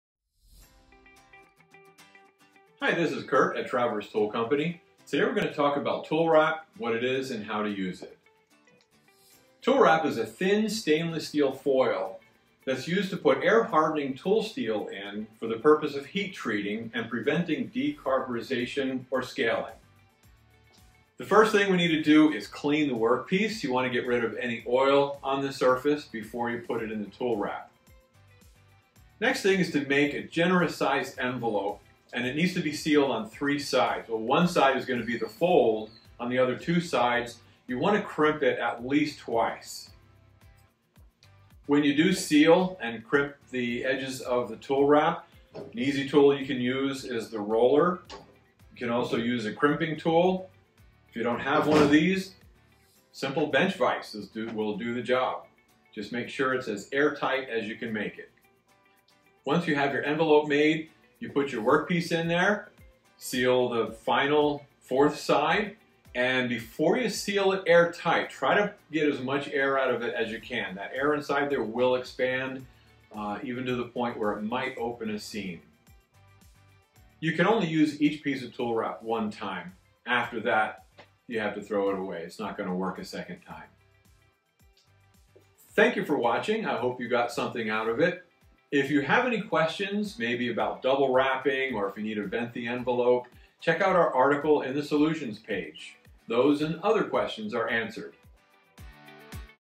What Is Tool Wrap And How Do You Use It?
Tool Wrap is a Stainless Steel .002" thick foil that is often used by tool & die makers, mold makers, machinists, and various maintenance personnel for wrapping tooling components in order to protect them from decarburization (or scaling) during the heat-treating process. Watch Kurt Repsher demonstrate the correct way to use tool wrap in this exclusive video.

TOOL WRAP BASICS

Use stainless steel Tool Wrap for tool steels that are air hardened. This heat-treating process is ideal when a controlled vacuum furnace is not available. It also saves time and expense by allowing the hardening process to be done in-house rather than having to send parts out for treatment.

HOW IS TOOL WRAP USED?Tool Wrap Roller

Select type based on heat treatment temperature: Type 321 Stainless Steel is used in temperatures up to 2000° F. Type 309 Stainless Steel will work in temperatures up to 2240° F.
Cut to size: Tool Wrap is made out of a material that can easily and quickly be cut with snips or heavy scissors.
Clean the tool surface: Any oil or residue left on the metal surface of the workpiece to be heat-treated should be cleaned off first.
Wrap the tool: Place the tool the folded Tool Wrap envelope, then carefully seal the open ends by folding and creasing it with a Crimper Tool at least twice.
Seal it tight: Remove as much air as reasonably possible before sealing, using a Foil Roller or Crimper Tool to help make a proper, airtight seam. The sealed Tool Wrap envelope will lock out the air, simulating the results of using a vacuum furnace. The part to be heat-treated doesn’t have to fit tightly in the Tool Wrap, but the envelope enclosing it should be as airtight as possible in order to prevent excess oxygen from entering, as that can cause pitting and scaling on the surface of the part. At the higher temperatures required, the Tool Wrap may stick to the workpiece during hardening, which is usually caused by either wrapping that’s too tight, the supporting surface being uneven, or a foreign substance like oil or residue that remained on the workpiece surface. Sometimes even if smooth work support surfaces are used and the part is “clean” and loosely wrapped, sticking can still occur.
Tech Tip: A liberally dusting the tool with aluminum oxide or magnesium oxide will fix this problem and help prevent the wrap from sticking to the tool.

WHEN SHOULD I VENT TOOL WRAP?

When wrapping a large, oddly shaped part: The expansion and contraction of a well-sealed Tool Wrap envelope is normal while heating and cooling because of the sealed-in air. With an exceptionally large or unusually shaped part, the trapped-in air could strain the Tool Wrap significantly enough to pierce through the foil. Most likely to occur during its cooling process, this isn’t usually a serious concern, and it can be eliminated completely, by venting the envelope. Prepare your Tool Wrap envelope in the usual way and leave about a 1/4" opening on one corner. Ensure the vent remains open, load the envelope into the hot furnace and the entrapped air will escape through the vent as the envelope heats—thus eliminating any pressure built up during the heating cycle. As soon as the envelope becomes hot, seal the vent corner by folding it over with a pair of tongs, and then allow the envelope to remain in the furnace until the heating cycle is completed. The venting procedure also reduces any pressure that would be caused by the rapid collapse of an expanded package during cooling.
After heat-treating, and prior to quenching: Removing the foil prior to quenching is not only difficult, but surface scaling and decarburization from exposure to the atmosphere may also occur—so the work piece doesn’t usually need to be removed from the Tool Wrap. However, the insulating effect of the Tool Wrap can sometimes make it necessary to remove it before quenching in order to obtain more rapid hardening.

TOOL WRAP TECH TIPS:

Cleaning or surface preparation may or may not be required after the heat-treating process. Due to oxidation by the entrapped air, a blue or tan surface color will be apparent. This discoloration is only cosmetic, however, and it can easily be removed by buffing if desired.
The Tool Wrap envelopes cannot be reused. The titanium content of Type 321 or 309 Stainless Steel is a characteristic that makes it ideally suited for preventing decarburization. The titanium acts as a catalyst by reacting with any oxygen within the foil before the oxygen can react with the surface carbon in the tool. After use, the titanium is somewhat depleted, thereby offering no safeguard in preventing decarburization. In addition, the foil becomes very brittle after use, preventing the wrap from forming a tight seal and also making it more prone to tears.